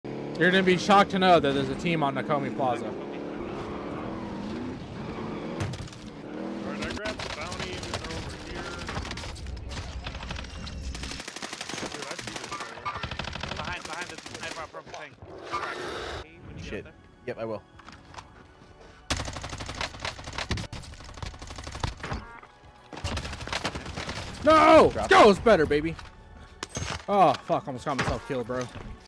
XM4 and Bullfrog is dope!
XM4 Buff to the smoothed out and combo with the bullfrog makes it a game changer!! Yall need to run this loadout!

You can catch me at the following below
- Live Tuesday - Saturday @ 6 pm (CST)